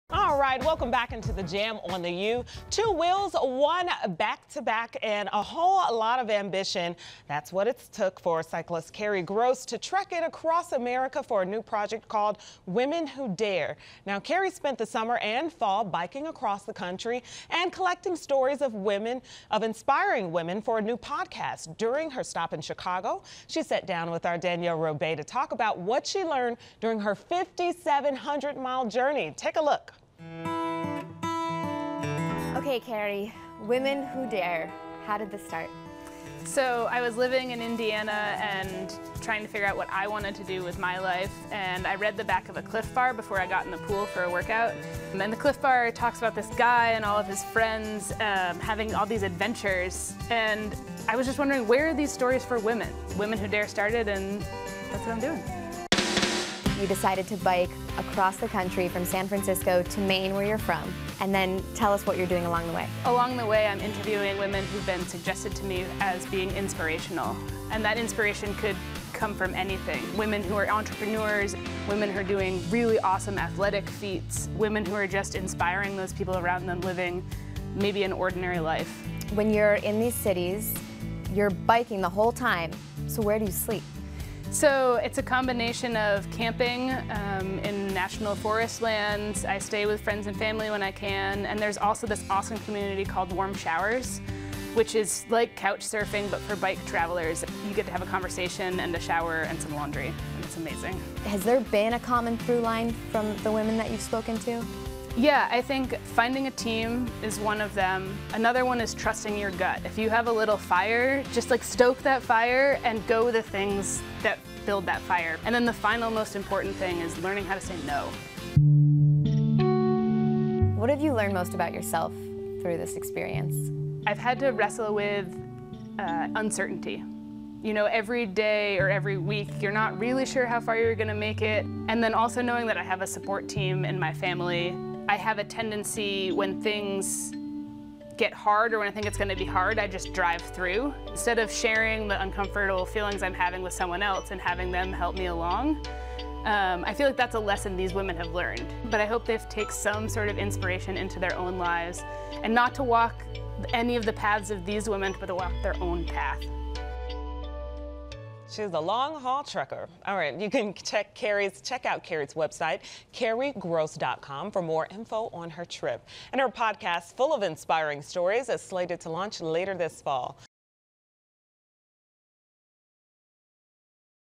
Women Who Dare: Cyclist Kerry Gross
5,700 MILES: Danielle interviews cyclist Kerry Gross trekked across America to collect stories of inspiring women.